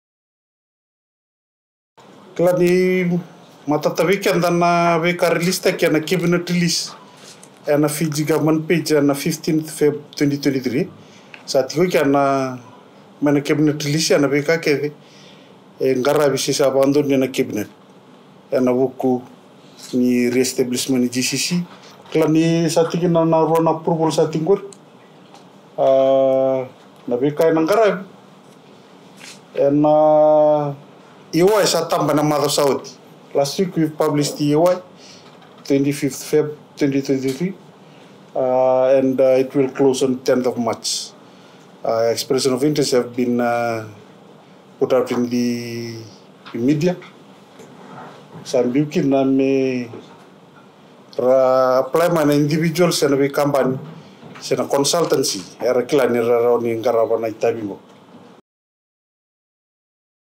Re-establishment of the GCC complex to begin in June and to cost $6 million | 1/3/23
The re-establishment of the Great Council of Chiefs complex in Nasese will commence in June and shall be completed by the end of next year.